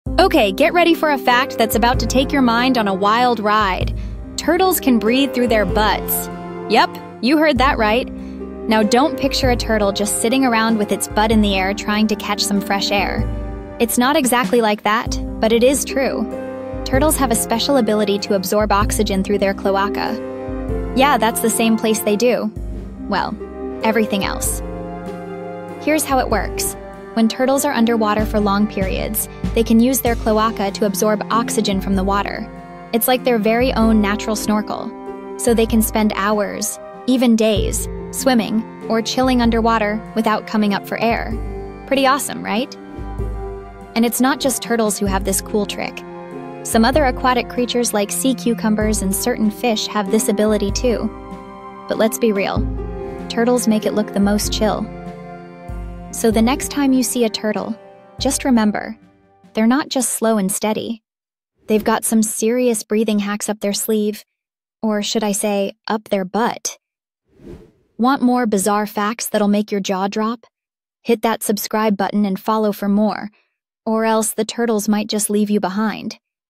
Turtles can breathe through their butts!
Turtles can breathe through their BUTTS! 🐢💨 Seriously, these guys have a natural snorkel system that lets them stay underwater for hours without coming up for air. You won’t believe how they do it—and it’s not as gross as it sounds! 😂  Want more mind-blowing facts like this? You never know what weirdness we'll uncover next!